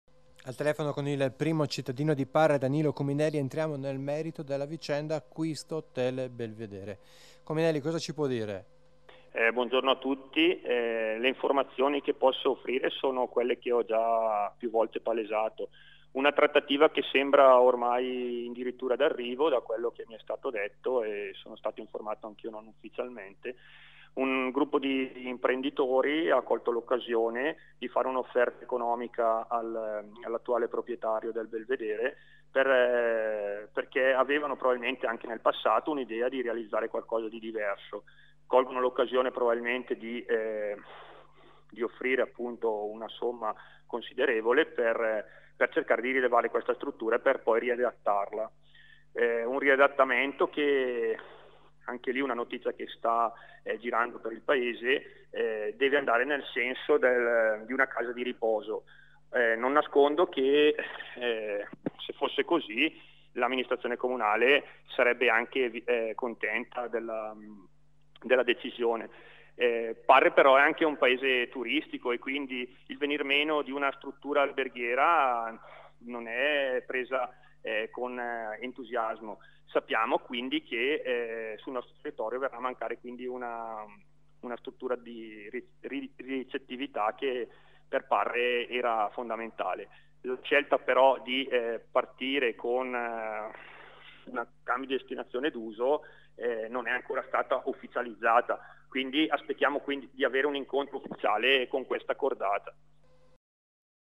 Parre, verso l'accordo per l'acquisto dell'Hotel Belvedere
A Parre le finestre dell'hotel Belvedere, struttura dove qualche settimana fa sembrava possibile l'arrivo di migranti, sono chiuse. All'ingresso, sulla vetrata della veranda, si legge anche il cartello con la scritta "chiuso". Sembra proprio che la cordata di imprenditori locali intenzionata ad acquistare l'immobile sia riuscita a definire l'accordo. Ma sentiamo questa intervista al primo cittadino Danilo Cominelli.